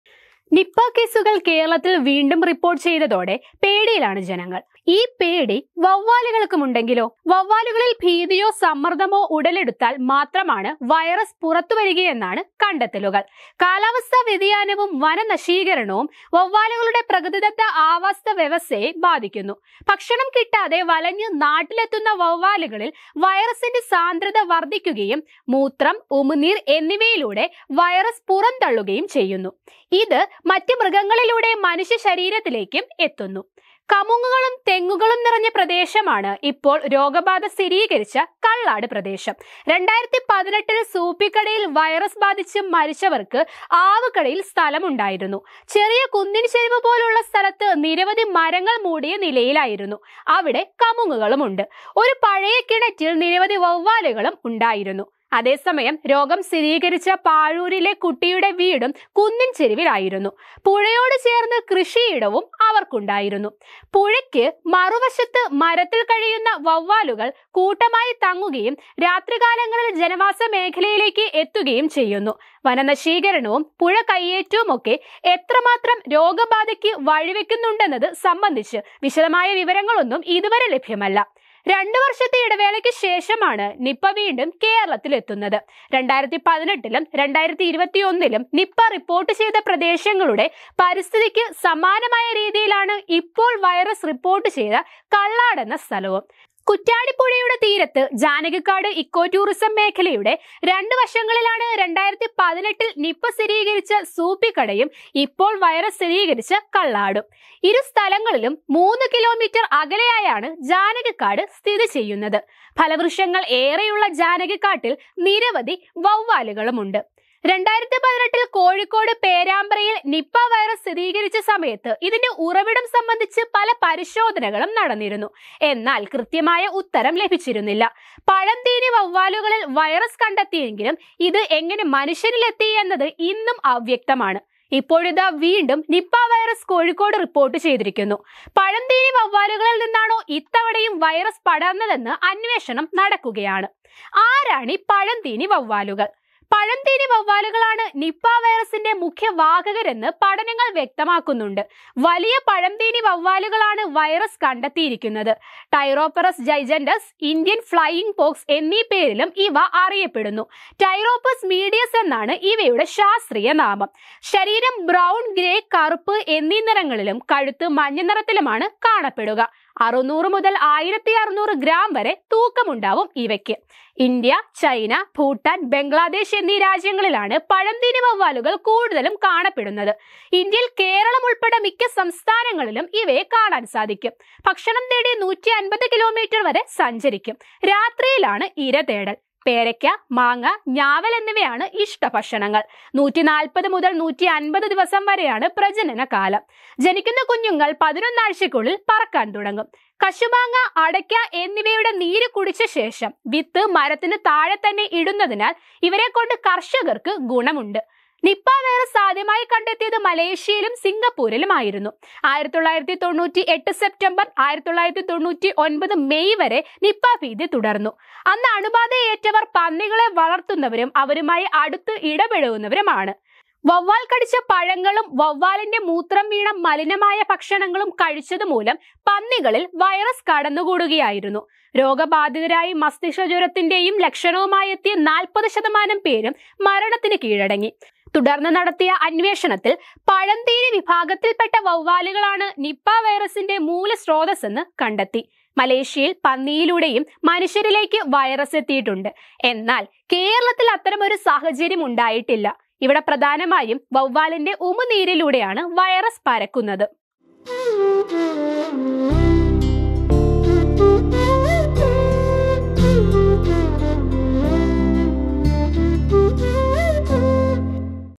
ആരാണ് പഴംതീനി വവ്വാലുകൾ‌ ? | Nipah | Indian Flying Fox
The findings show that the virus can only be released when bats are frightened or stressed. Climate change and deforestation are affecting the natural habitats of bats. The concentration of the virus increases in bats that come home without food and excrete the virus through urine and saliva.| വവ്വാലുകളിൽ ഭീതിയോ സമ്മർദമോ ഉടലെടുത്താൽ മാത്രമാണ് വൈറസ് പുറത്തുവരികയെന്ന് ആണ് കണ്ടെത്തലുകൾ. കാലാവസ്ഥാ വ്യതിയാനവും വനനശീകരണവും വവ്വാലുകളുടെ പ്രകൃതിദത്ത ആവാസവ്യവസ്ഥയെ ബാധിക്കുന്നു. ഭക്ഷണം കിട്ടാതെ വലഞ്ഞ് നാട്ടിലെത്തുന്ന വവ്വാലുകളിൽ വൈറസിന്റെ സാന്ദ്രത വർധിക്കുകയും മൂത്രം, ഉമിനീര് എന്നിവയിലൂടെ വൈറസ് പുറന്തള്ളുകയും ചെയ്യുന്നു.

"നാടിനൊപ്പം നേരിനൊപ്പം"